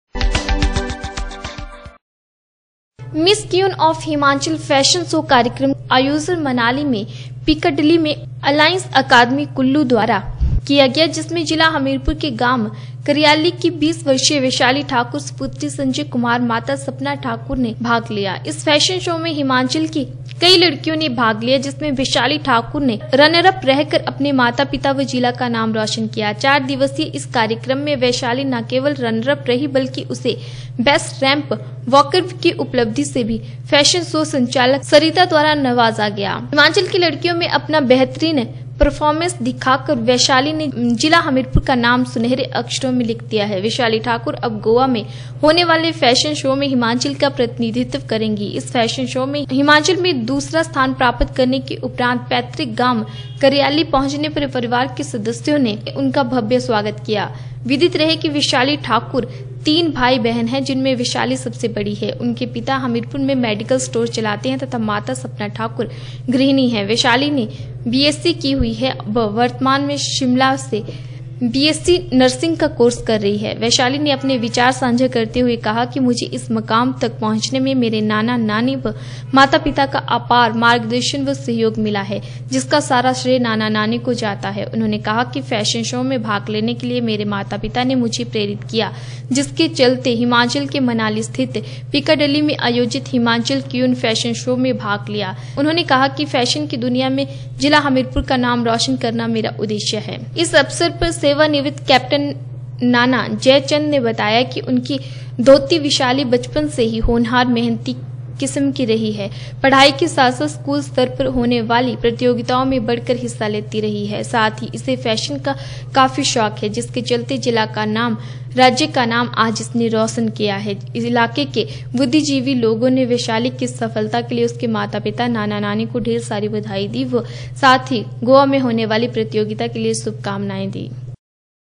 जिला हमीरपुर के गांव करयाली की २० वर्षीय वैशाली ठाकुर मिस क्यून
मिस क्यून ऑफ हिमाचल फैशन शो  कार्यक्रम का आयोजन मनाली में पिकाडली में एलांइस अकादमी कुल्लू द्वारा १८ नबंवर २०१७ को आयोजित किया जिसमें जिला हमीरपुर के गांव करयाली की २० वर्षीय वैशाली ठाकुर सुपुत्री संजय कुमार माता सपना ठाकुर  ने भाग लिया । इस फैशन शो में हिमाचल की २८ लडकि यों ने भाग लिया ।  जिसमें वैशाली ठाकुर ने रनरअप रह कर अपने माता पिता व जिला का नाम रोशन किया है । चार दिवसीय इस कार्यक्रम में वैशाली न केवल रनरअप रही बल्कि उसे बैस्ट रैंप वॉकर की उपाधि से भी फैशन शो संचालक मैडम सरिता द्वारा नवाजा गया।